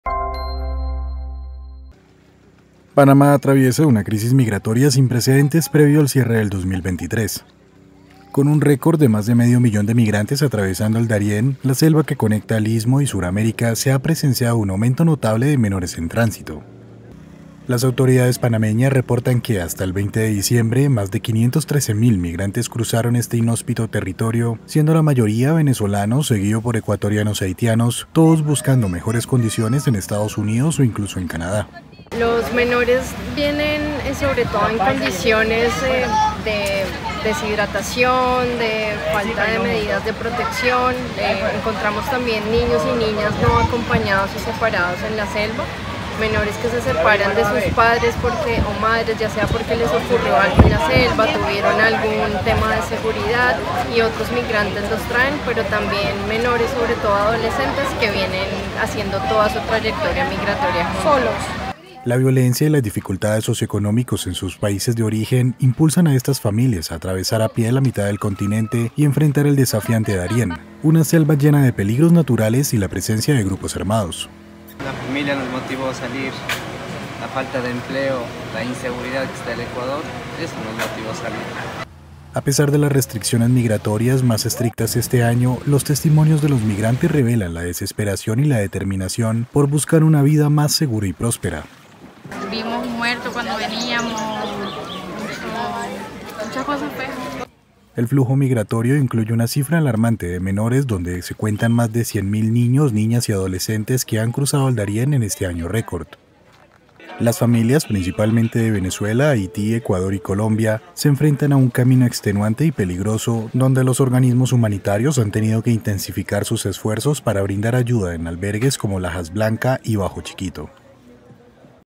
Panamá adivierte aumento récord de migrantes y menores en tránsito en el 2023
Panamá cierra este 2023 marcado por la agravada crisis migratoria: este año se ha batido el récord de medio millón de migrantes en tránsito por el Darién, la selva que comunica el istmo y Suramérica, una cifra que duplica el registro del año pasado y abarca marcado un aumento de menores.